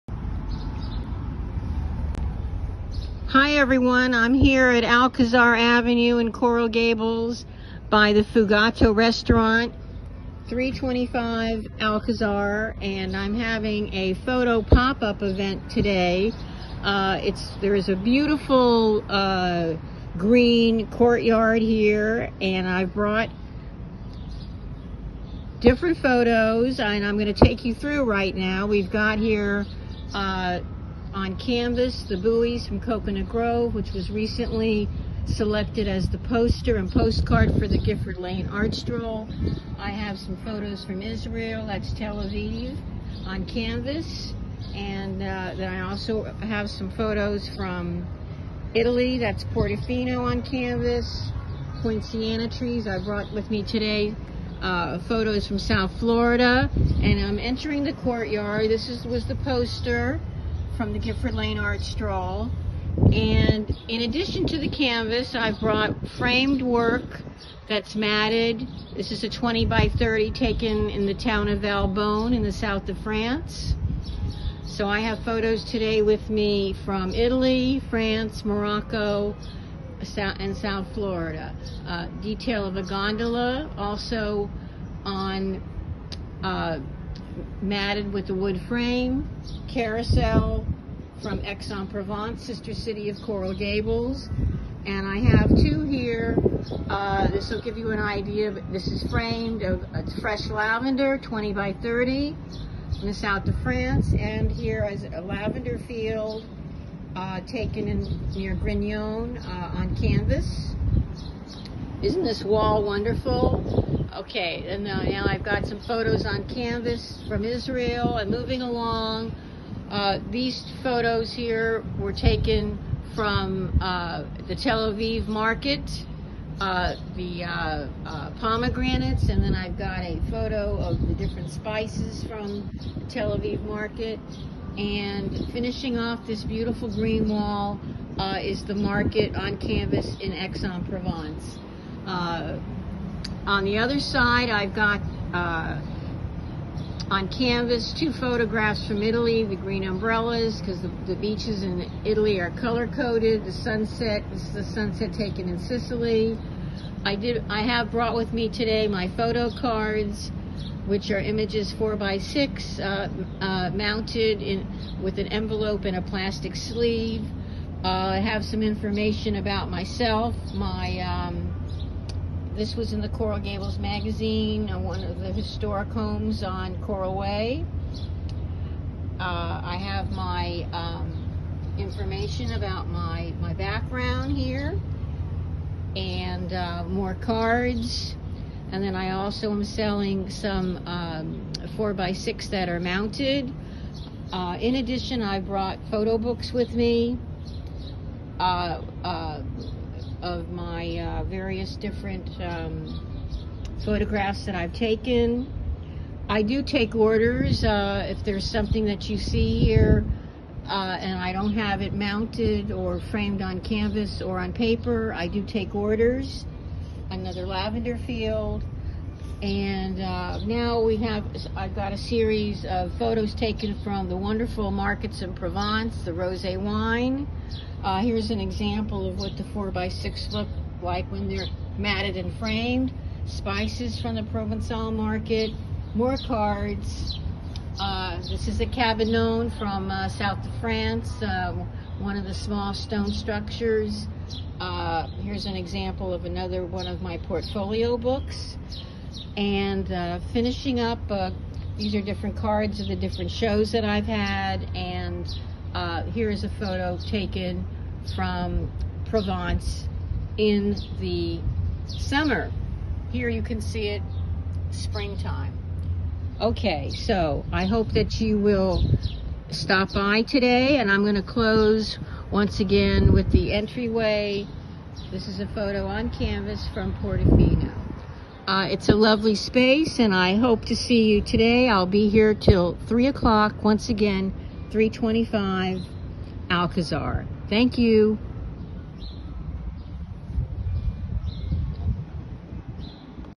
Photo Travels with Alice
I had a wonderful opportunity to do a pop-up show in the courtyard of Fugato Fusion Cuisine on Saturday,  April 23.